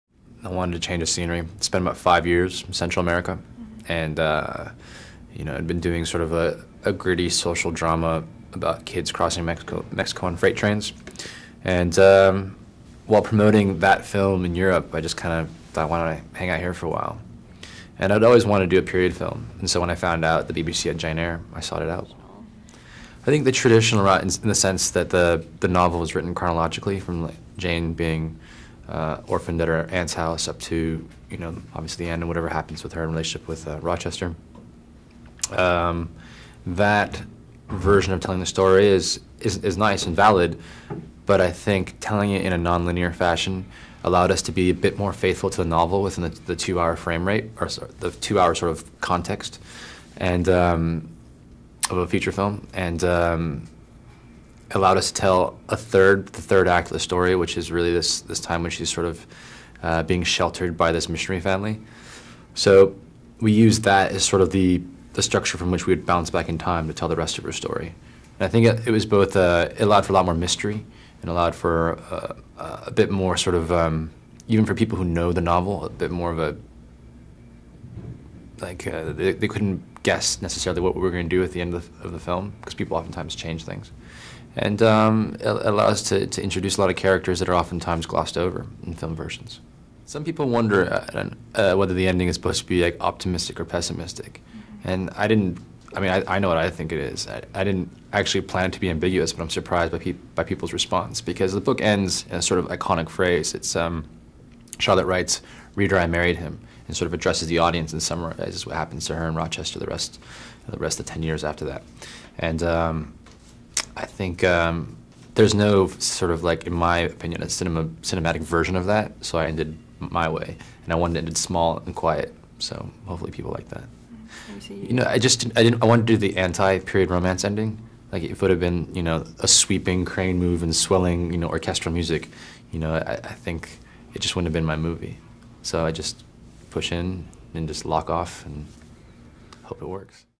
Cary Fukunaga for Jane Eyre
Director Cary Fukunaga's thoughts on filming Jane Eyre.